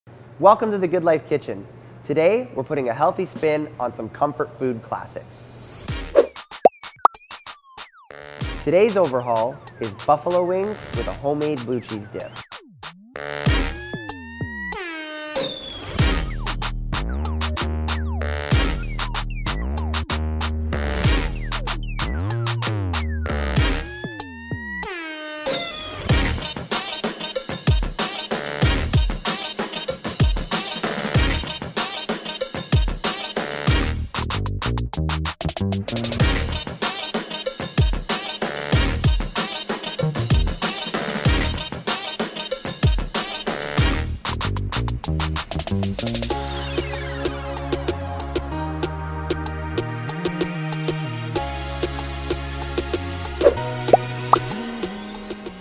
GoodLife Kitchen: Gets Comfy! Wings Edition
Make your favourite comfort food healthier with this recipe for rings!